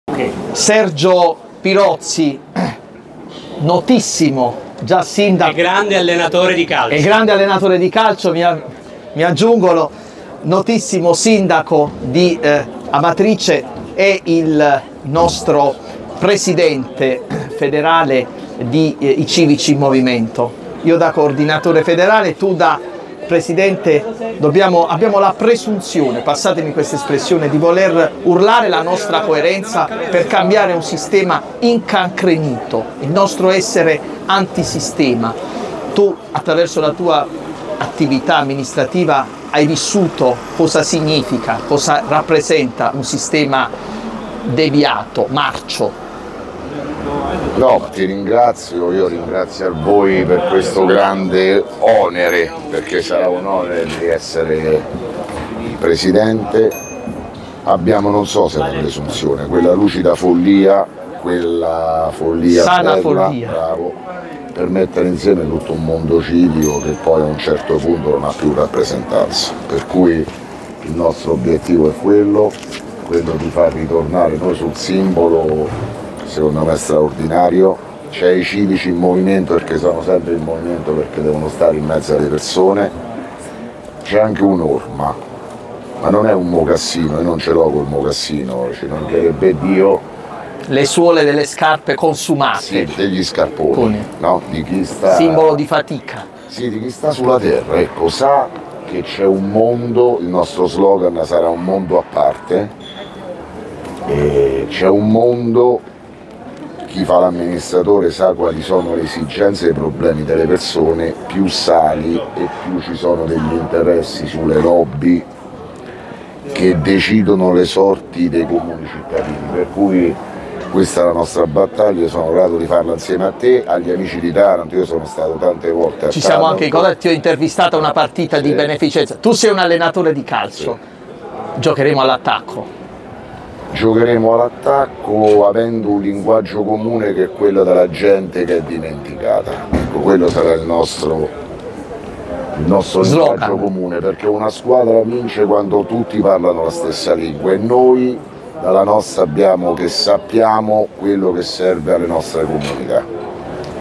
"I CIVICI IN MOVIMENTO": I DIRIGENTI NAZIONALI LUIGI ABBATE E SERGIO PIROZZI
"I Civici in Movimento", la grande coalizione nazionale che unisce le varie realtà civiche locali, di cui sono Coordinatore federale: eccomi con Sergio Pirozzi, Presidente federale del Movimento ed ex sindaco di Amatrice, noto per il suo impegno legato al dramma del terremoto che colpì la sua città e le zone vicine.